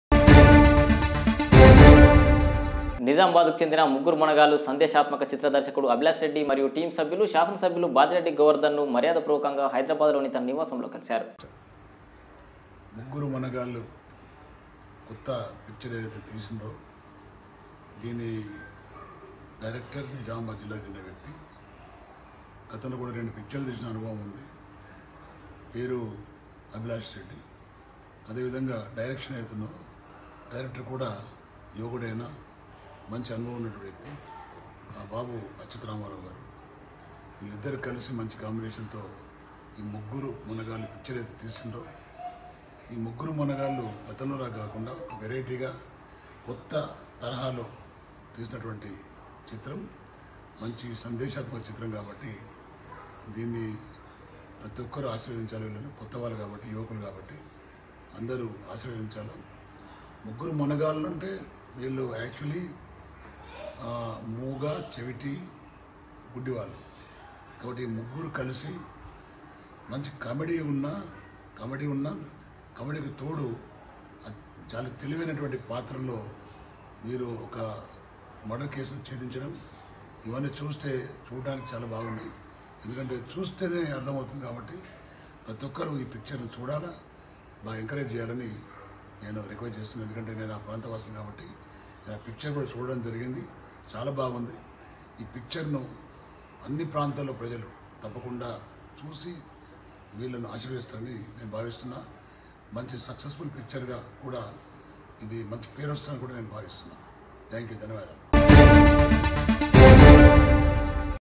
ప్రతి ఒక్కరు చూడాల్సిన మంచి సందేశాత్మక చిత్రం "ముగ్గురు మొనగాళ్లు|TS24 News
రాష్ట్ర రాజధాని హైదరాబాదులో ఆయన నివాసంలో చిత్రానికి సంబంధించిన యూనిట్ సభ్యులు  శాసనసభ్యులు  బాజిరెడ్డి గోవర్దన్ ను  మర్యాదపూర్వకంగా కలిశారు..
సినిమాను గౌరవ శాసనసభ్యులు తిలకించిన అనంతరం ఆయన అభిప్రాయాన్ని వెల్లడించారు..
ముగ్గురు మొనగాళ్ళు చిత్రం మూగ చెవిటి గుడ్డి వాళ్ల పాత్రలను హాస్యం జోడించి మరియు తెలివిగా చిత్రాన్ని నిర్మించారని, ప్రతి ఒక్కరు ఈ చిత్రాన్ని ఆదరించాలని చెప్పారు..
నిజామాబాద్ జిల్లా ప్రజలందరూ సినిమాను తప్పకుండా ఆదరించాలని అన్నారు..
నిజామాబాద్ యువకుడు దర్శకుడు అభిలాష్ రెడ్డి అతి చిన్న వయస్సు గల వ్యక్తి, చిత్ర పరిశ్రమలో నిజామాబాద్ జిల్లా ఖ్యాతిని, జిల్లా గర్వించదగ్గ వ్యక్తిలా ఎదగాలని ఆకాంక్షించారు..
రెండు ప్రాంతాల ప్రజలు ముగ్గురు మొనగాళ్లు చిత్రాన్ని ఆదరించి, యువ దర్శకుల కథలను మరింత ఆదరించాలని కోరారు..